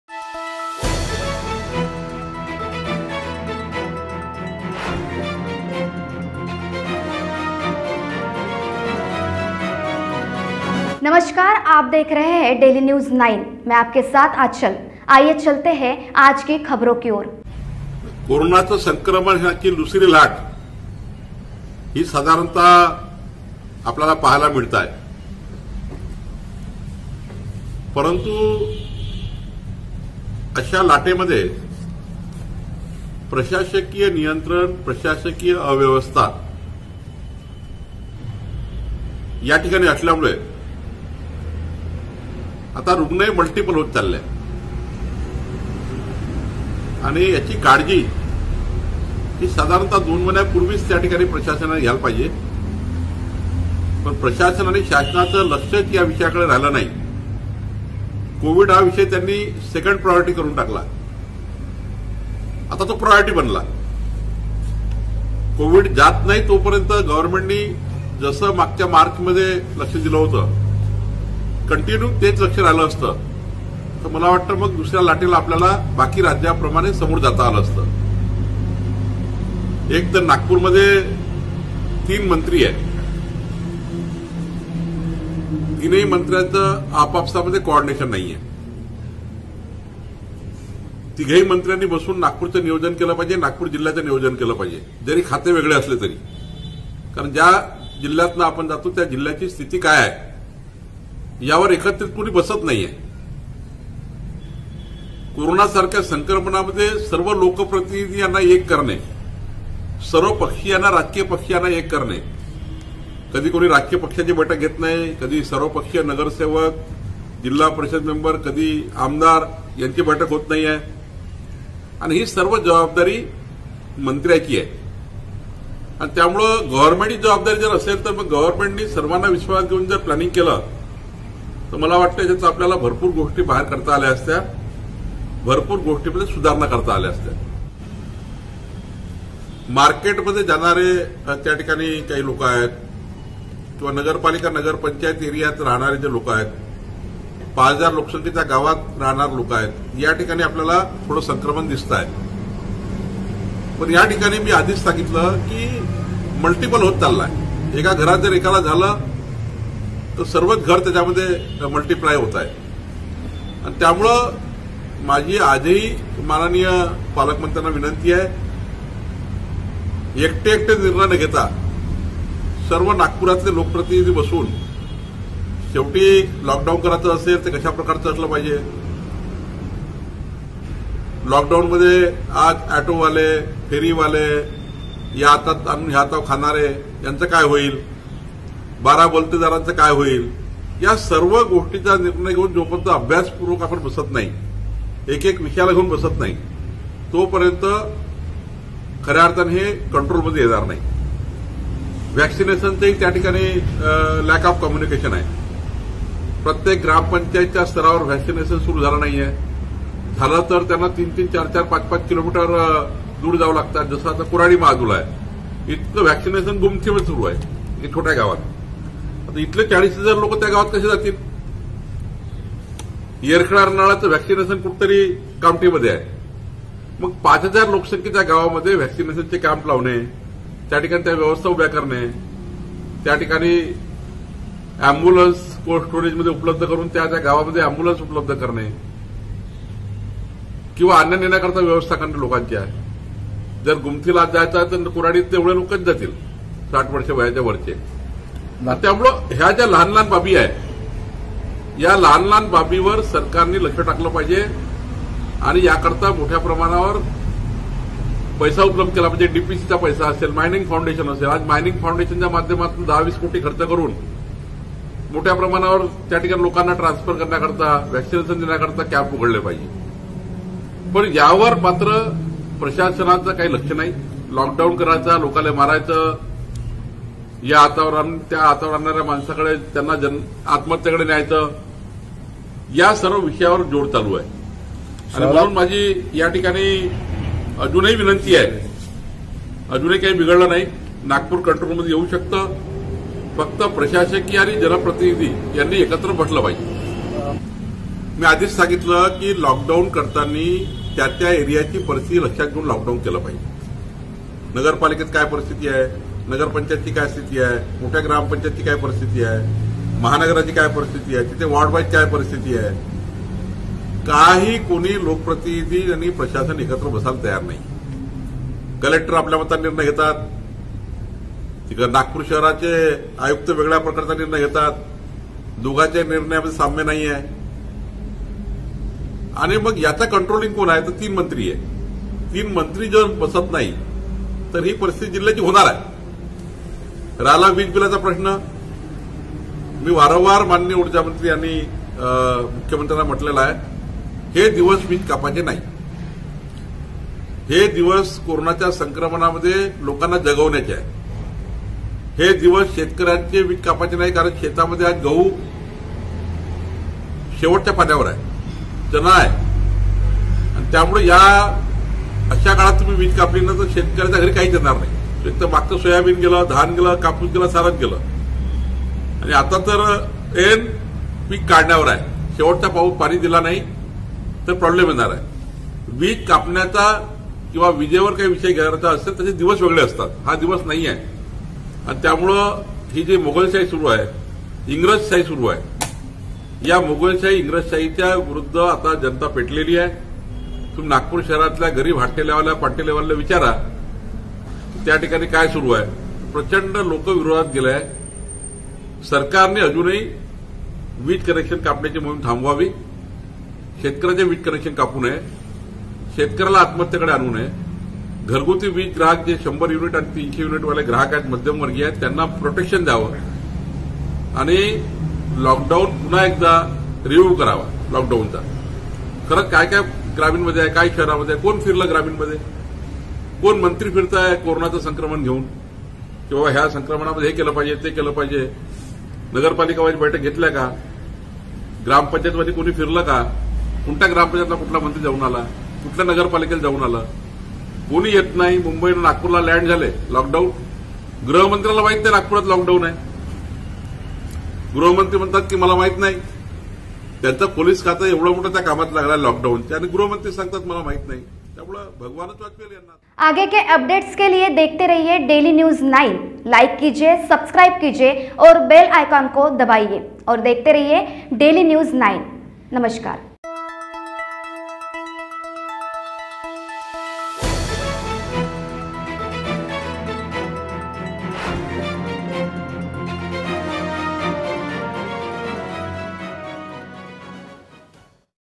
daily news 9 कोरोनाचे संक्रमण ही दुसरी लाट लॉकडाऊन हे पर्याय नाही चंद्रशेखर बावनकुळे
कोरोनाचे संक्रमण ही दुसरी लाट लॉकडाऊन हे पर्याय नाही चंद्रशेखर बावनकुळे daily news 9 ki report